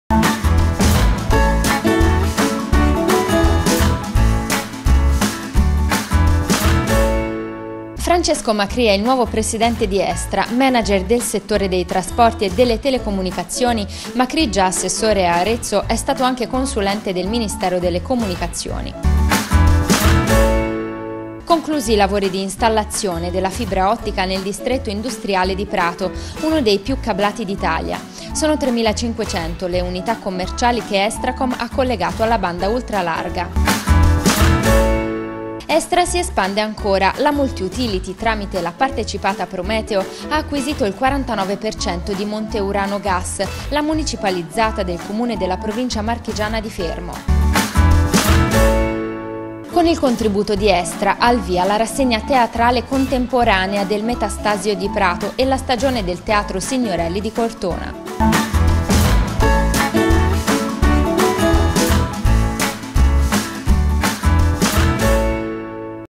Estra Notizie Ottobre 2016
Francesco Macrì, nuovo presidente di Estra
Estracom porta la fibra ottica al distretto industriale di Prato
Estra si espande acquisendo il 49% di Monte Urano Gas
Estra sostiene la cultura e il teatro